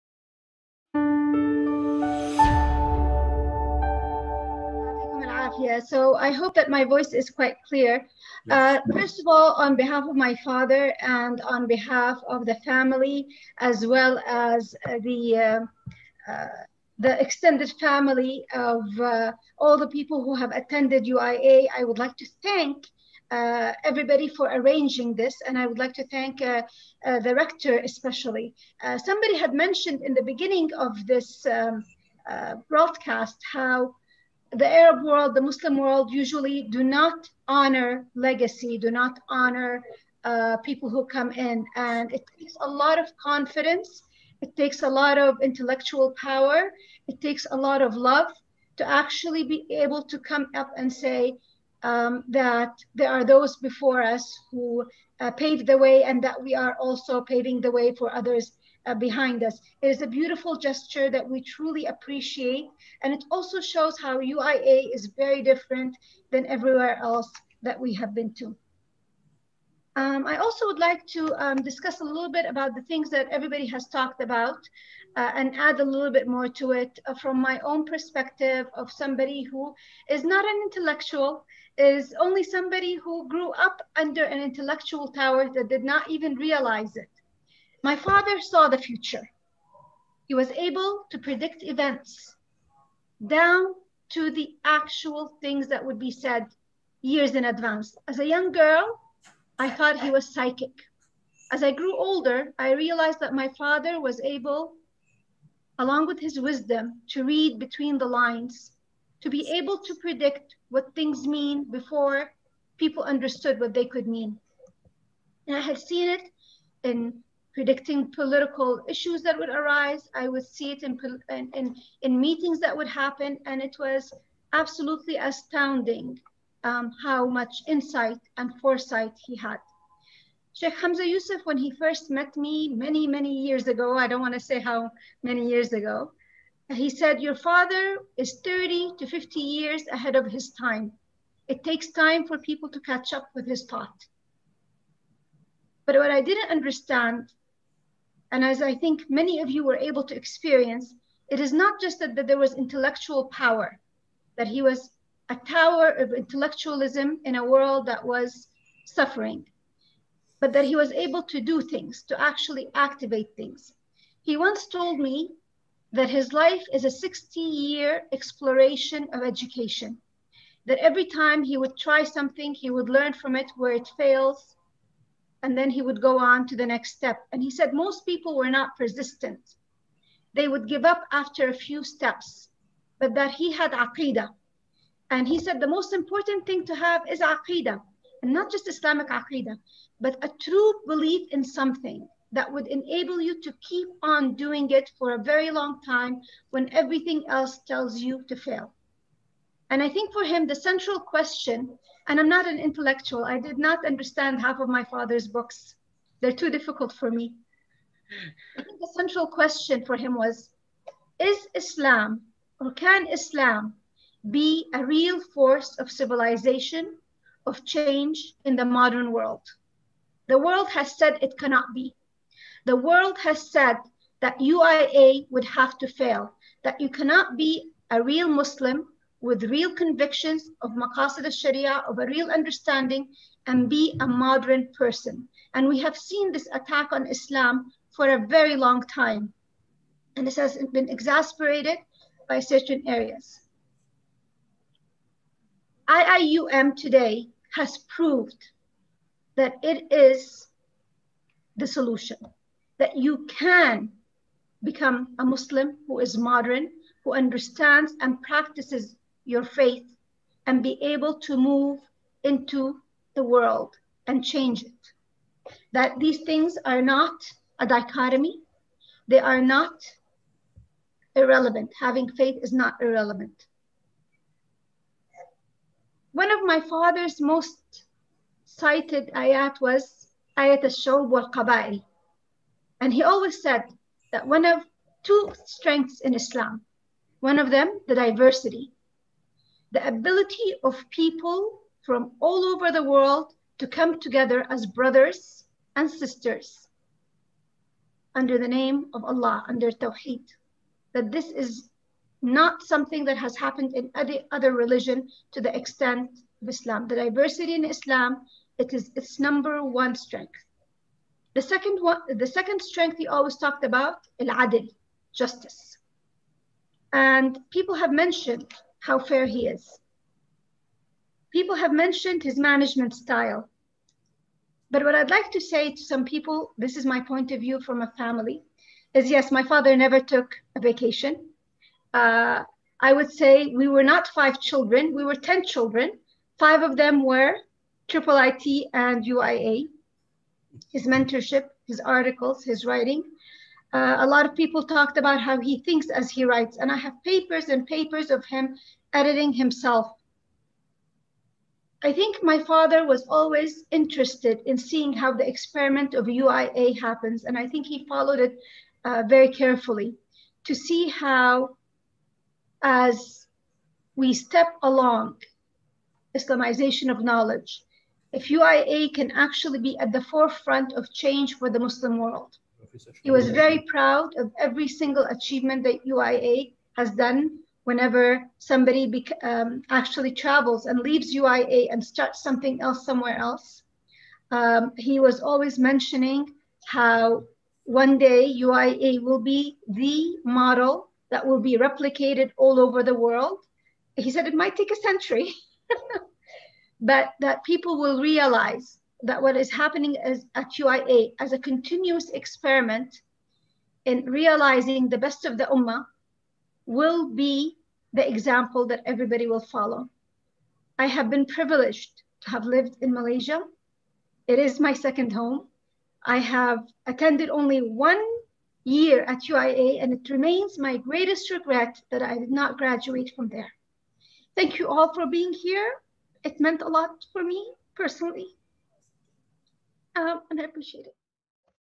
Prof. Emer. Dato' Dr. Abdulhamid to Islamic Thought & Education: Ms. Mona Abusulayman
Contributions of Professor Emer. Dato' Dr. Abdulhamid A. ABuSulayman to Islamic Thought & Education

Speaker:
Ms. Mona Abusulayman

19 Mar 2021 (Friday)
09:00 PM (Malaysia)